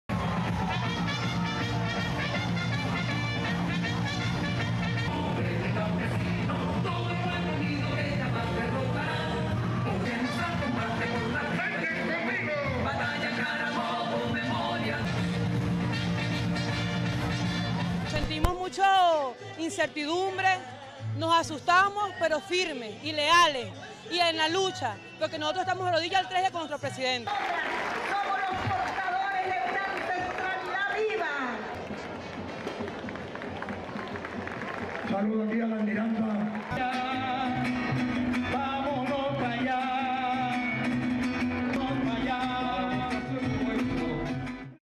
Venezuelans hold rally in support of Maduro after "attack"
Thousands of people gather in Caracas in support of Venezuela President Nicolas Maduro after an alleged "assassination" attempt was made on his life using an explosive-laden drone.